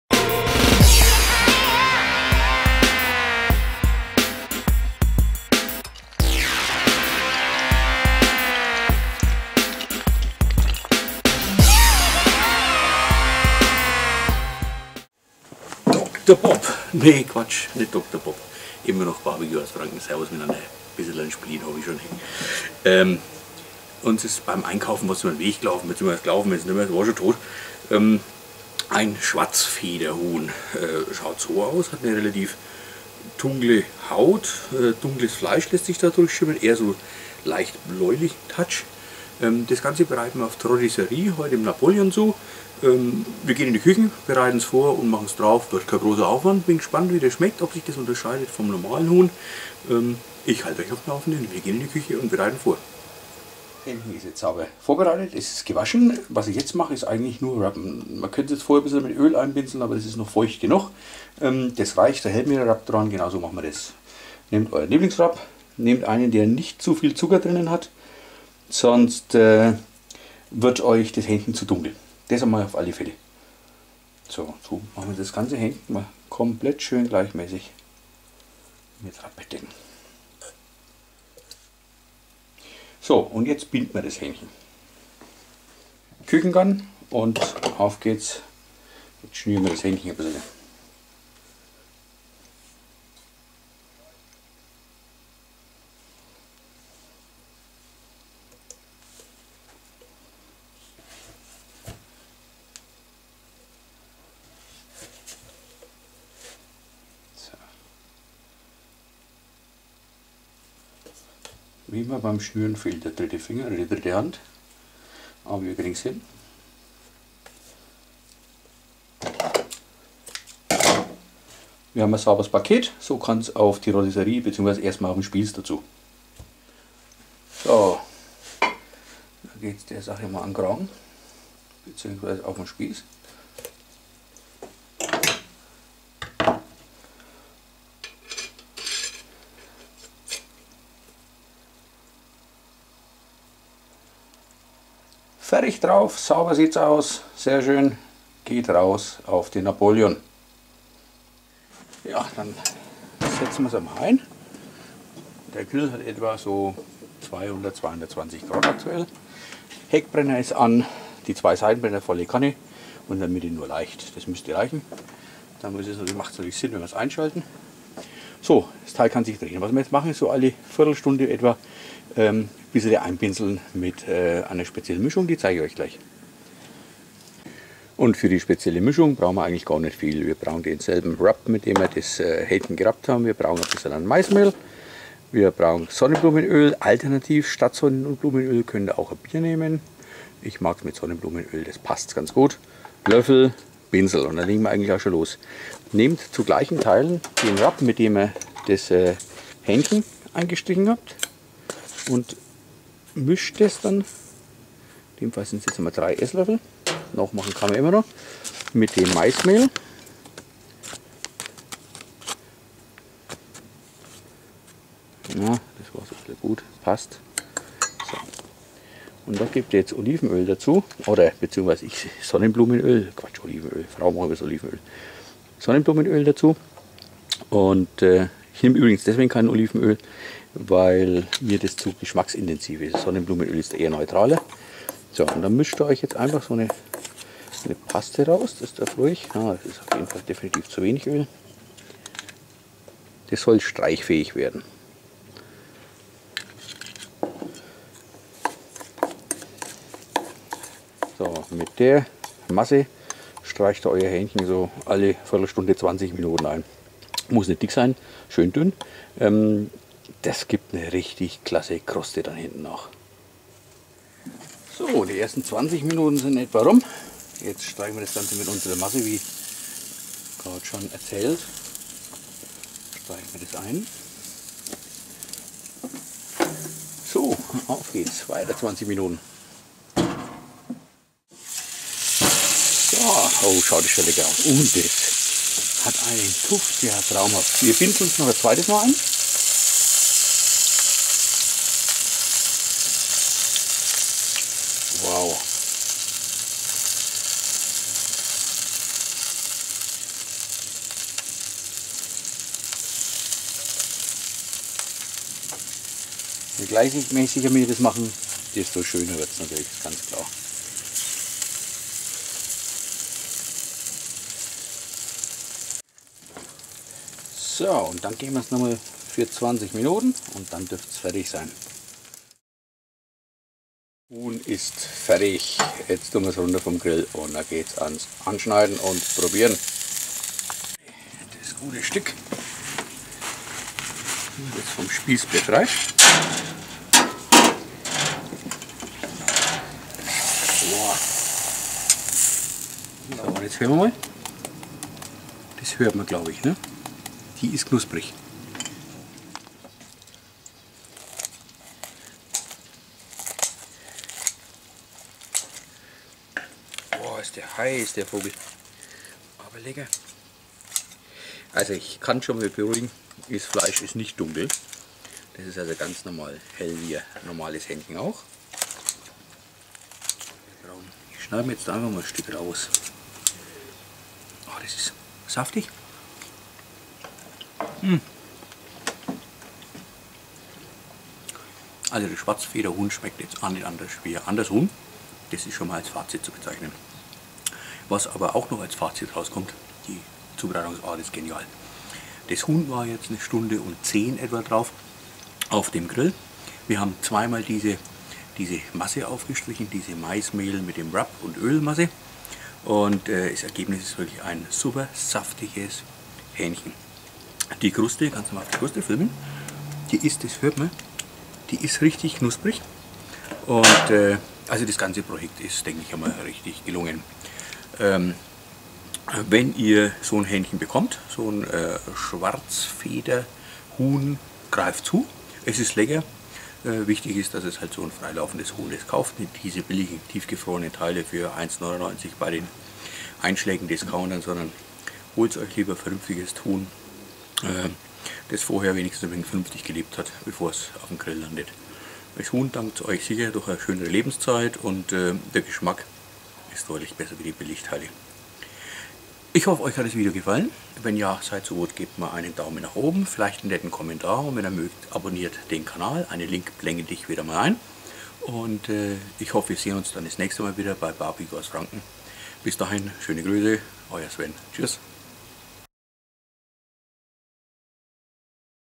Chicken auf der Napoleon Rotisserie zubereitet.
Ein Schwarzfederhuhn mit einer Maismehl Kruste.
Wenig Aufwand und super lecker.